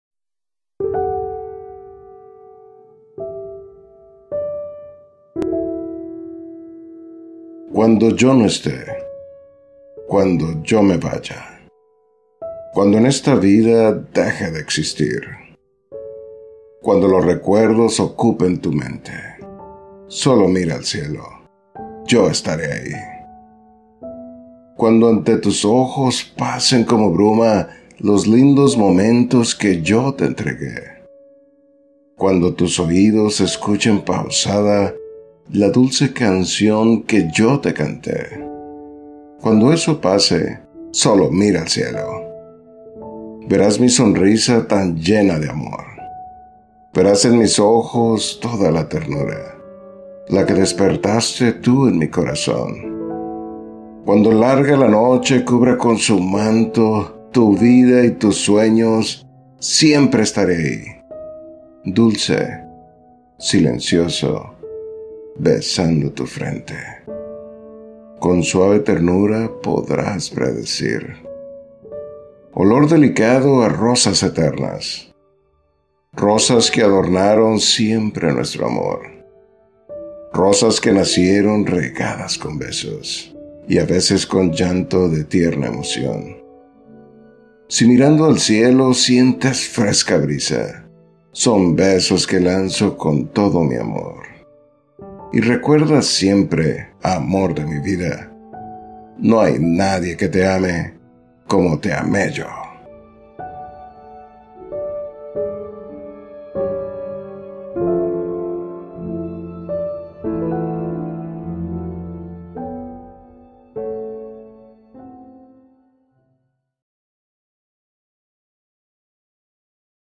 Mirando Al Cielo - Poema de Rosa Andrade
Mirar al cielo y saber que esa persona que nos amó esta ahi...
Autora: Rosa Andrade (Maby)
Voz: @ELMentadoChavez